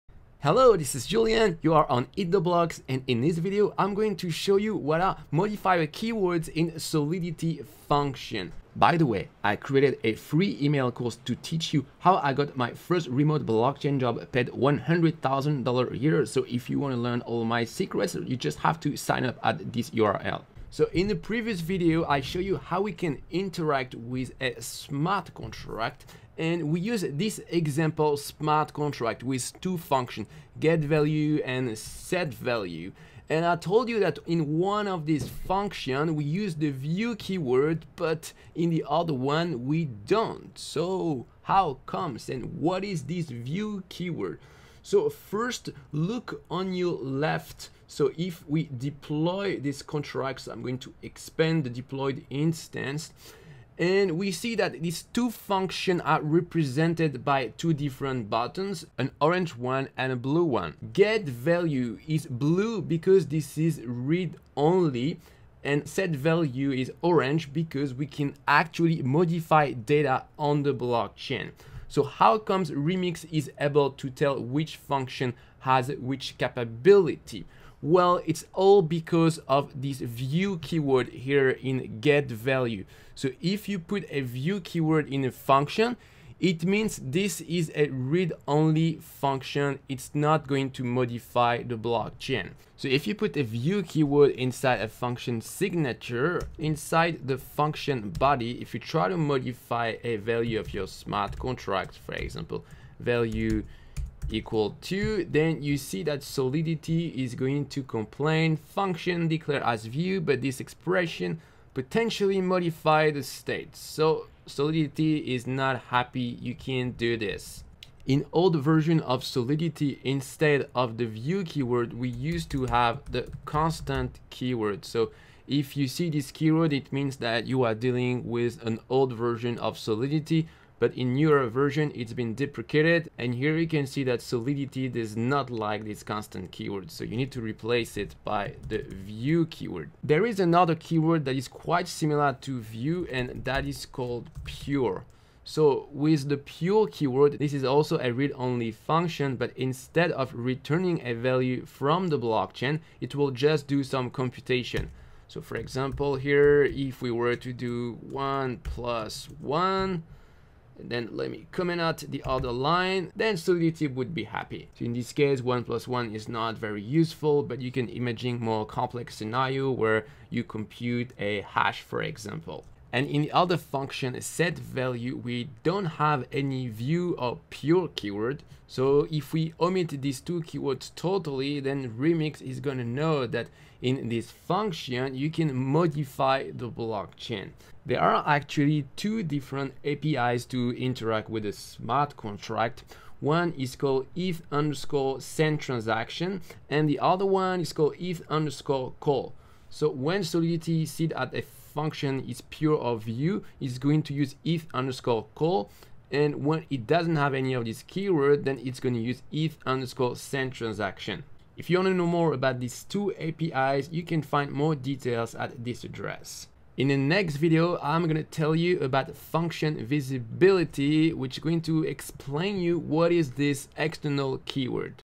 Solidity Tutorial: Function modifier keywords (view, pure, constant)
Register to the FREE mini-course "become a blockchain developer" on eattheblocks.com to learn how to get a remote blockchain job making 100k. (Like I did myself).

In this video I am going to explain you why some functions in Solidity have the view, pure or constant keywords.